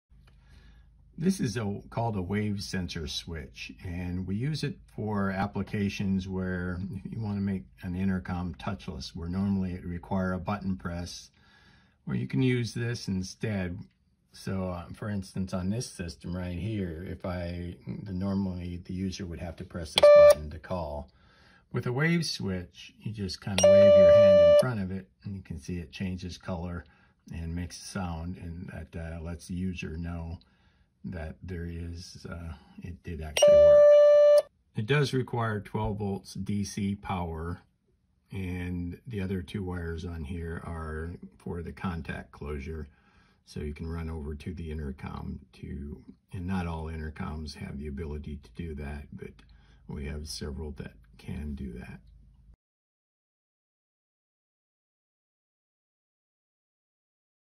Touchless Intercom Using a Wave Switch
The Covid-19 breakout has created more of a need for touchless products and using this wave sensor switch we can create a touchless intercom system for visitors at your door.

While this touchless switch is usually used for activating automatic doors, we have found it also works well for activating the call button on an intercom without having to touch the intercom's call button.

When a visitor arrives at the door they simply wave their hand in front of the sensor and the light on the unit will change from blue to green.